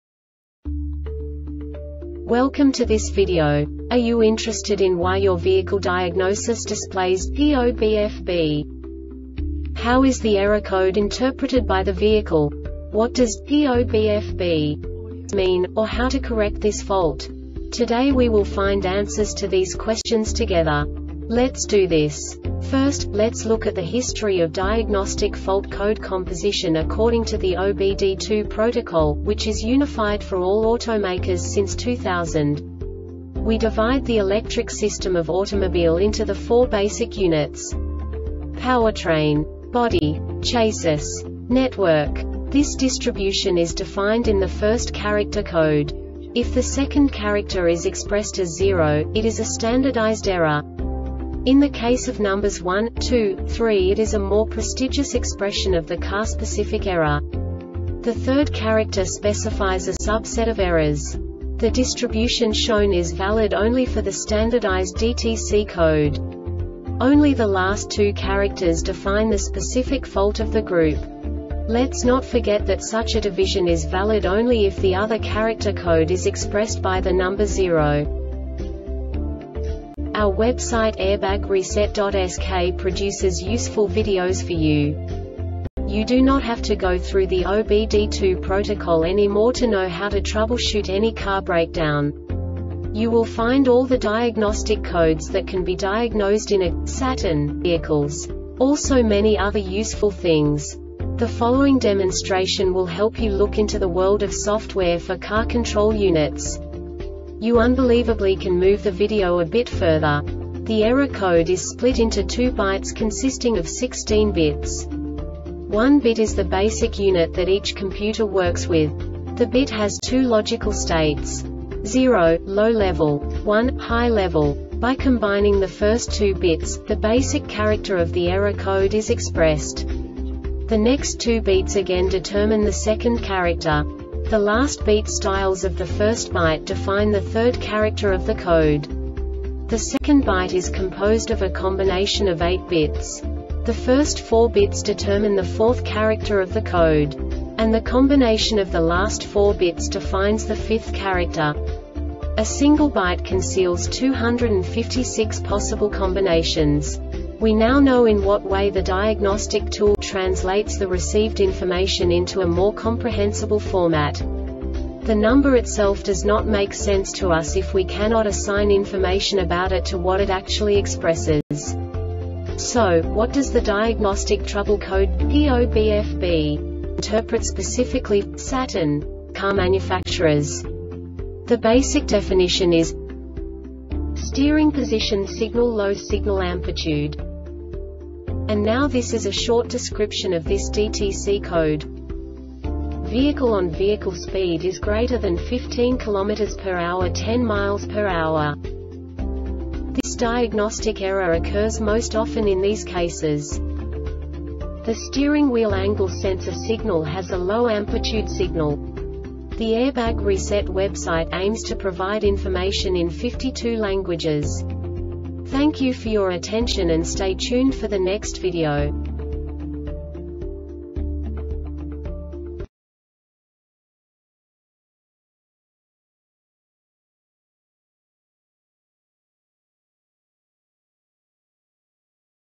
DTC Saturn P0BFB Short Explanation
Contents:
0:21 Basic DTC analysis according to OBD2 protocol standard.
1:48 Insight into programming
2:58 A diagnostically understandable description of the Diagnostic Trouble Code
3:05 Basic error definition

Make:
Saturn
Code:
P0BFB
Definition:
Steering Position Signal Low Signal Amplitude
Description:
Vehicle ON.Vehicle speed is greater than 15 km or h (10 MPH).
Cause:
The steering wheel angle sensor signal has a low amplitude signal.